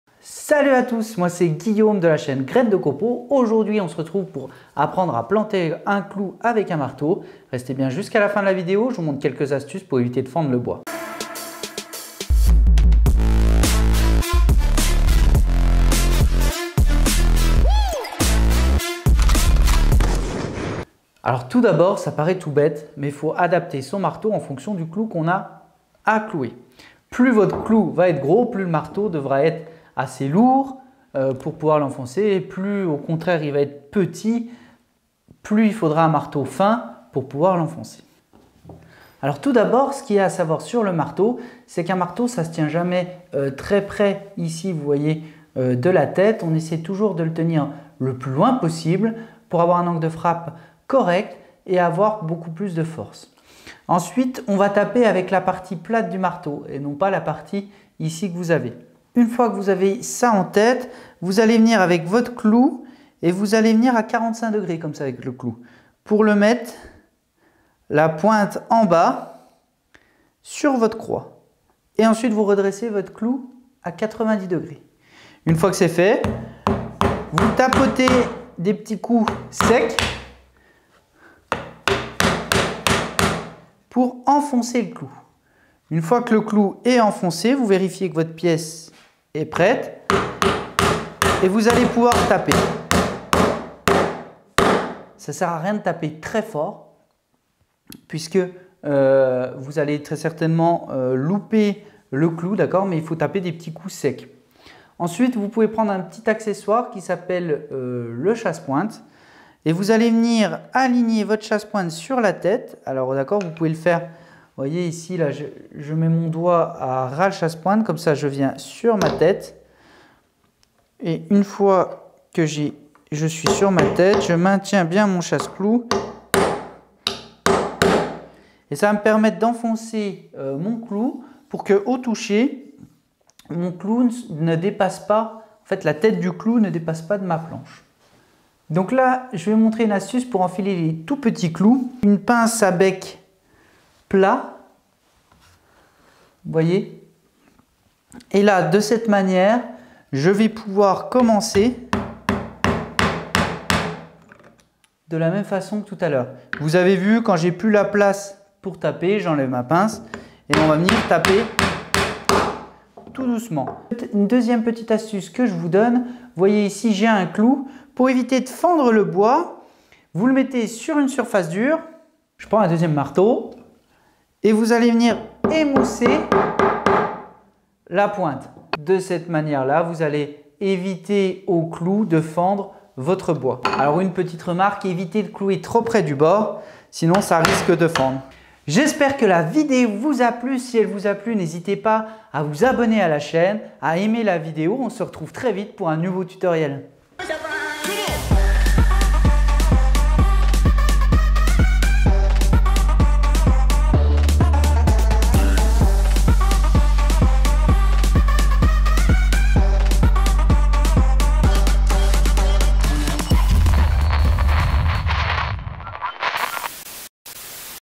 Comment planter un clou (ou pointe), sans fendre le bois. TUTO DIY +ASTUCES
Dans cette vidéo je vous explique comment planter un clou (Aussi appelé pointe) simplement et facilement. Par la même occasion je vous apprend à vous servir d'un marteau.
C'est un tutoriel simple (DIY) sur le bricolage et notamment sur les bases du bricolage à savoir planter un clou en sécurité et sans se faire mal.
Je vous donnes également 3 astuces en fin de vidéo.